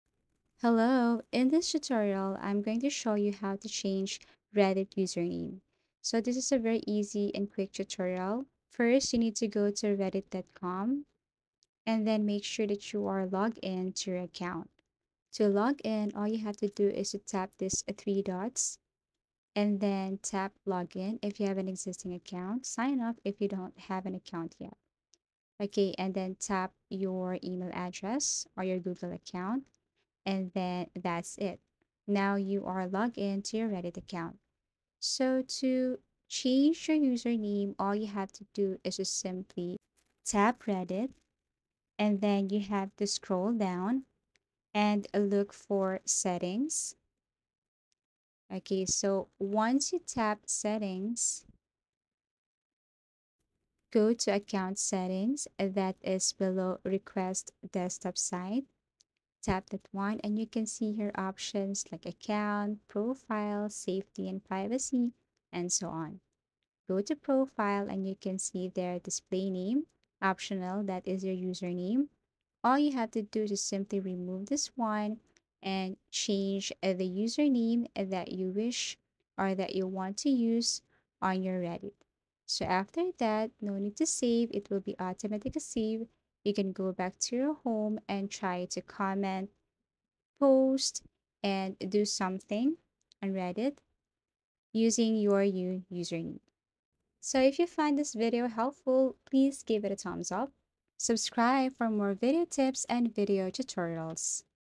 How To Change Reddit Username
How to Change Reddit Username: A Step-by-Step Guide

If you're looking to update your Reddit persona, you've come to the right place! Learn how to change your Reddit username effortlessly with this step-by-step guide.

1. Log in to Your Account: Access your Reddit account by entering your credentials. Make sure you are logged in to the account whose username you wish to change.

2. Click on User Profile: Once logged in, navigate to the top-right corner of the Reddit homepage. Click on your profile icon to access your user profile.

3. Go to User Settings: From the drop-down menu, select "User Settings." This will take you to a new page with various options related to your account.

4. Choose "Change Username": Scroll down on the User Settings page until you find the option "Change Username." Click on it to initiate the process.

5. Verify Your Account: Reddit will prompt you to verify your account for security purposes. Follow the on-screen instructions to complete the verification process.

6. Enter a New Username: After verification, you will be prompted to enter your desired new username. Choose a unique and memorable name to represent your Reddit identity.

7. Confirm Your Selection: Double-check the new username to ensure it is correct. Once you are satisfied, click "Confirm" to finalize the change.

8. Username Successfully Changed: Congratulations! Your Reddit username has been changed. Your new username will now be visible on your profile and throughout the Reddit community.

Please note that Reddit allows users to change their username only once. Therefore, choose your new username wisely, as you won't be able to change it again after this process.